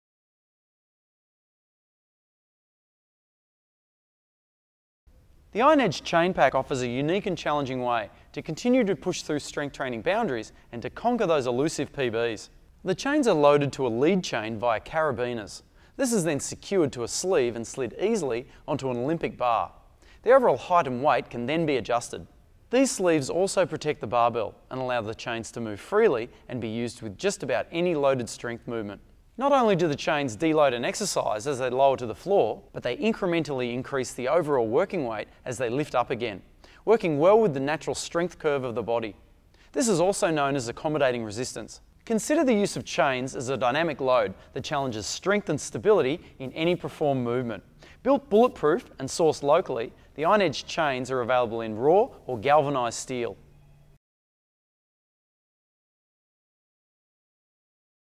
Iron Edge Chains – Product Video
The science and technicality behind chains in the gym is almost secondary to the fact that they look and sound like they belong in a high-performance space. The research is there; chains engage stabiliser muscles, train athletes to move more explosively, and enable the manipulation of the power curve. There's no denying that chains are simply badass.

Providing great training variety and specialised benefits, the Iron Edge Chain Pack offers a complete solution with simplicity and ease of adjustment.  The Iron Edge chain pack features specially designed sleeves allow you to effortlessly attach the supplied carabineers with any number of the 6 included chain lengths.